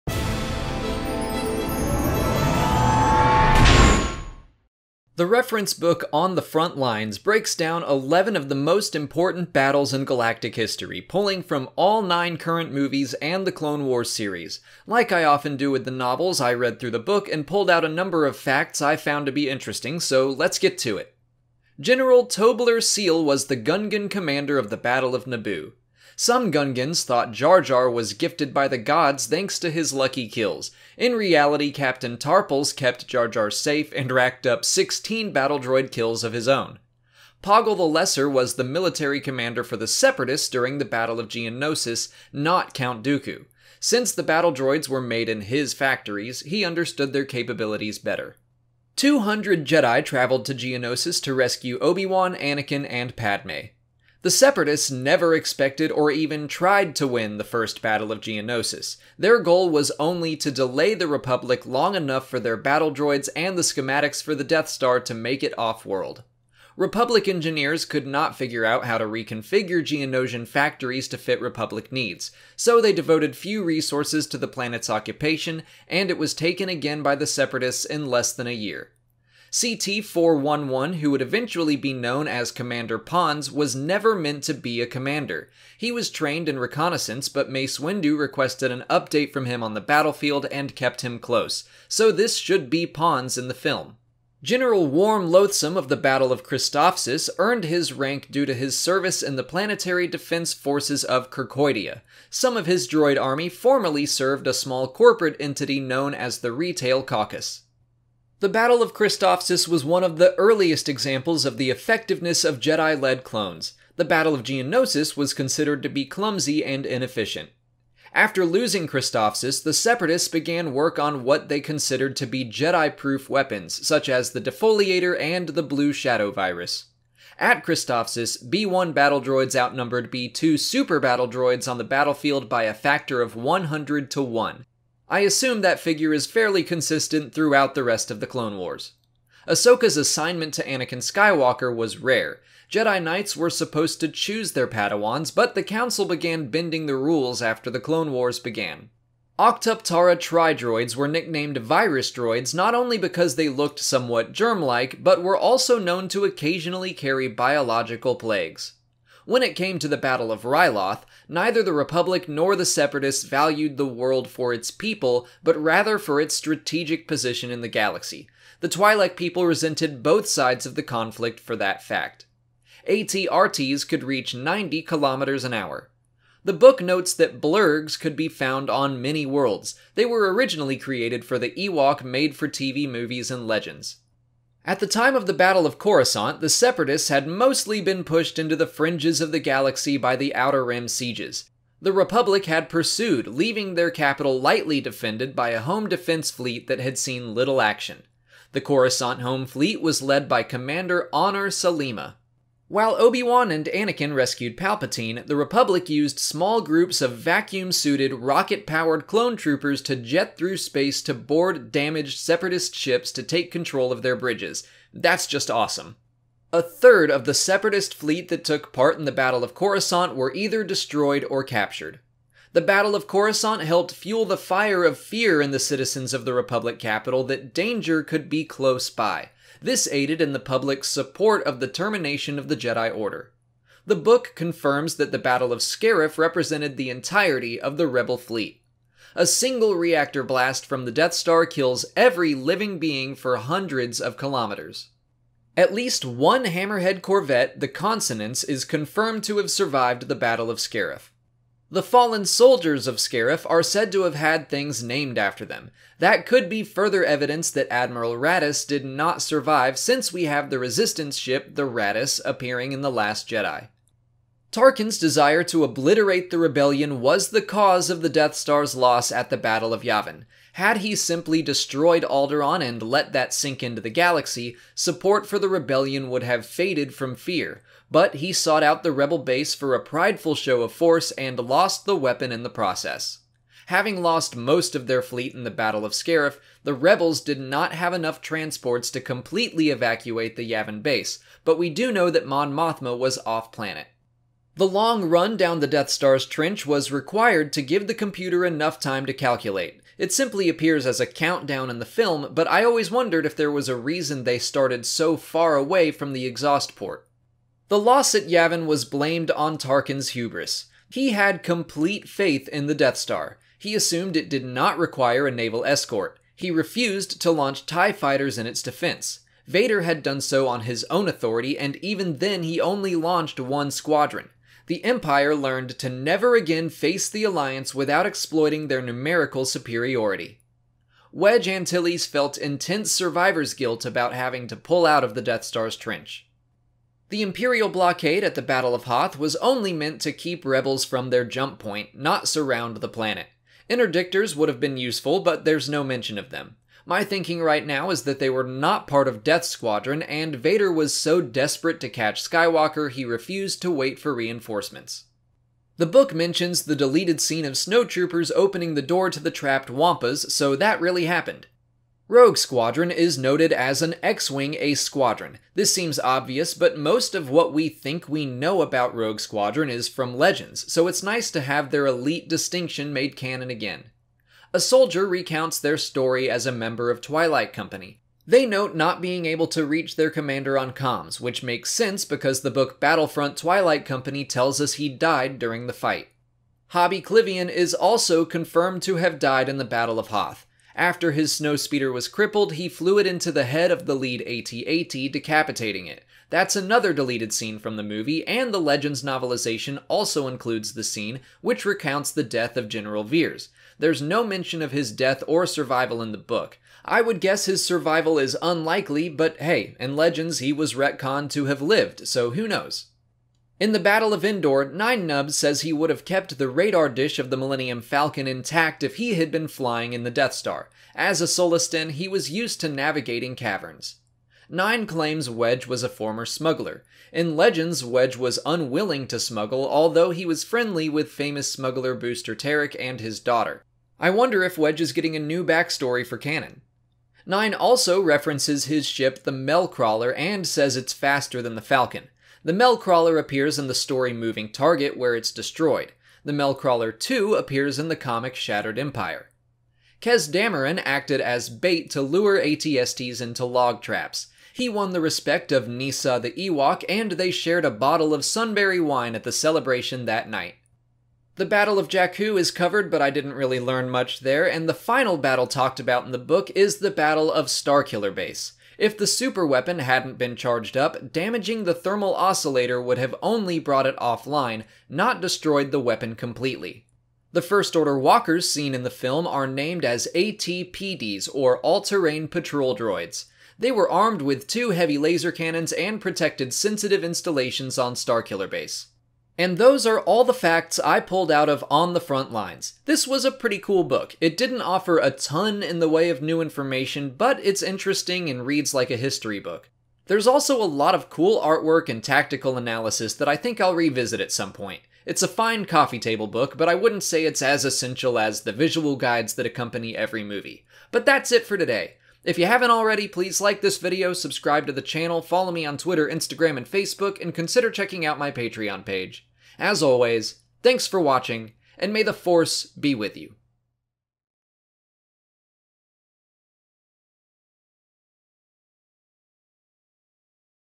40 Facts from Star Wars: On the Front Lines - Trivia, Connections, New Information, and More!
Here are all the interesting pieces of trivia from the reference book Star Wars: On the Front Lines.

Adam Amaya
Matthew Simon
Kroptar Draven
Nick Laureta

Jake Pickering
Kent Lundblad
Danny Stephens
Stephen Raleigh
Rhys Davies
J.S. Skye
Lord Shadow Raider